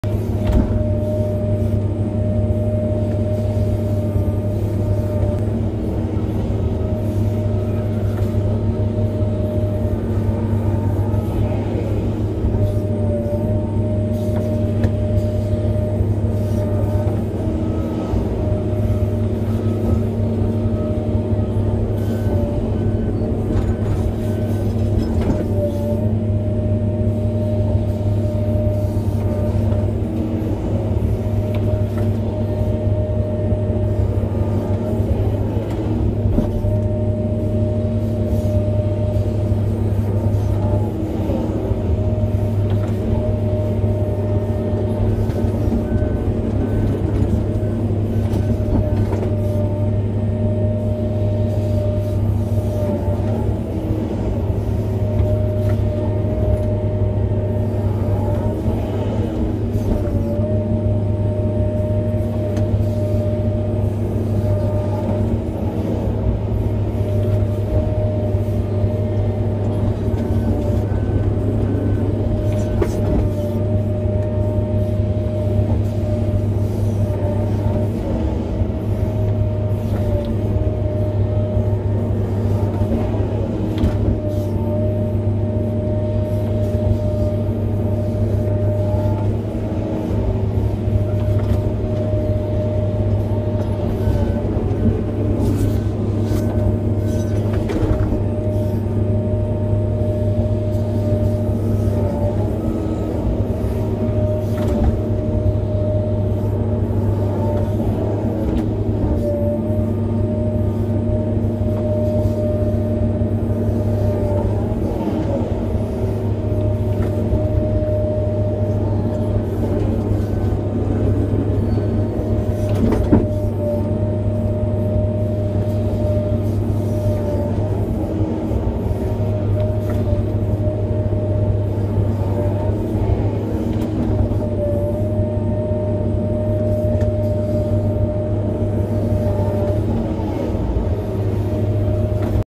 Shading Pipeline from Ditch Side
When the ditch is too wide to reach across to shade, I find this technique works best. Straight bulking the whole time, no messing around trying to stay level ... Lots of guys like to sit over the pipe on the dirt and pull it in. I find doing it that way, you waste a lot of time trying to stay level from the windrows you leave behind.